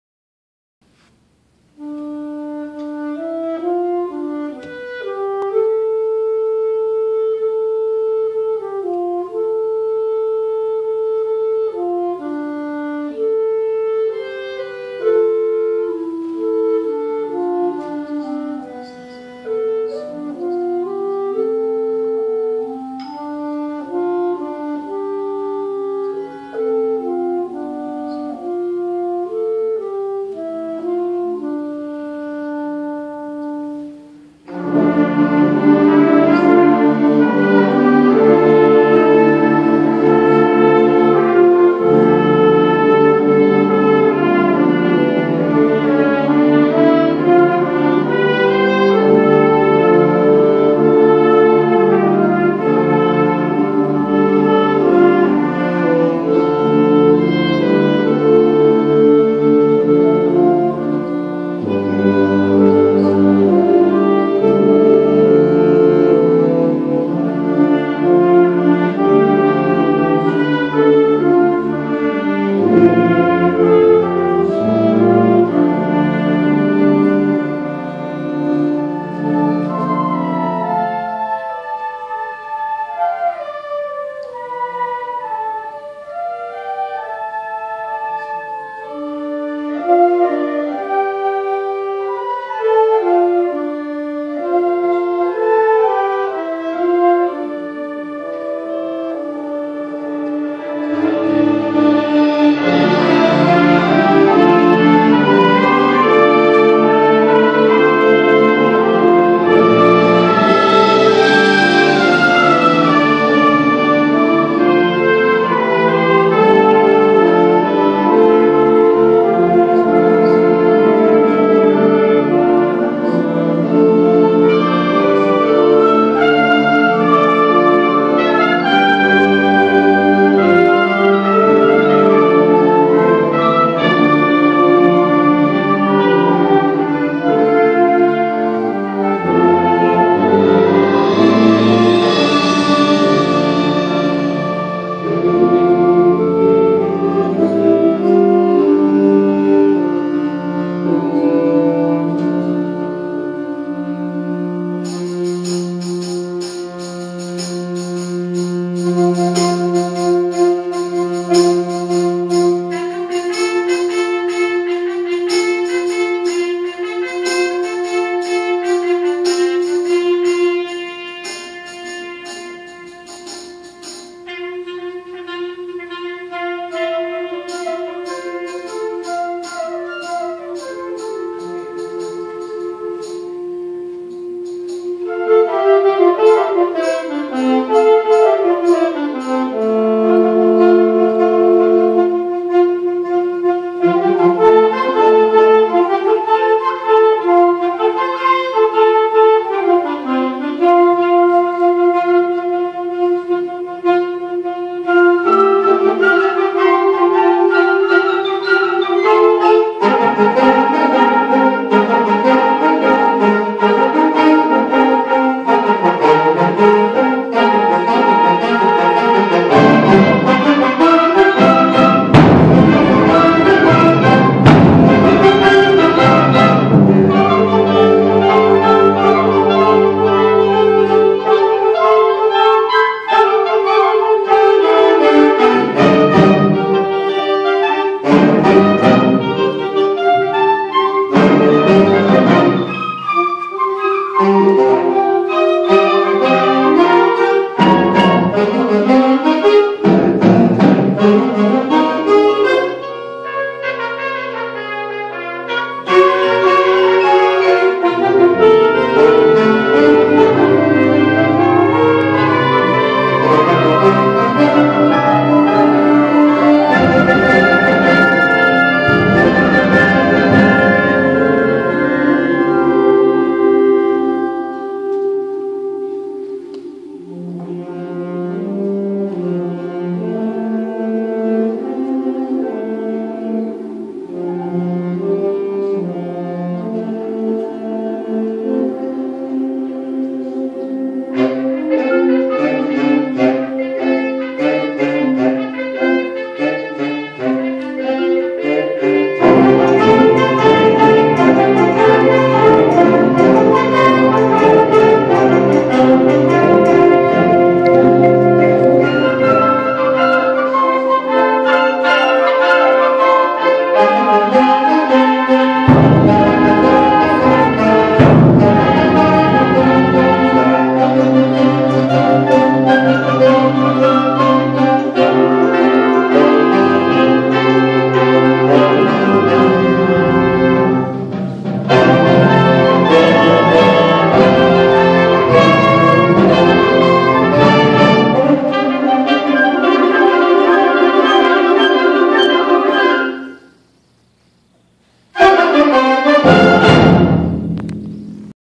Del Mar High School Symphonic Band
Symphonic competition at 2014 Feste Del Mar.